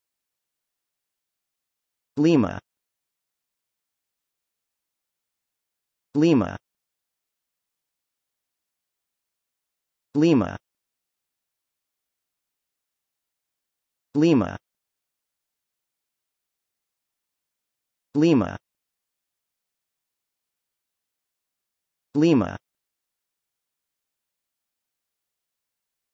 How To Pronounce Lima - How To Say: American pronunciation
Learn how to correctly say Lima in English with Free Pronunciation Tutorials. How To Pronounce. American pronunciation.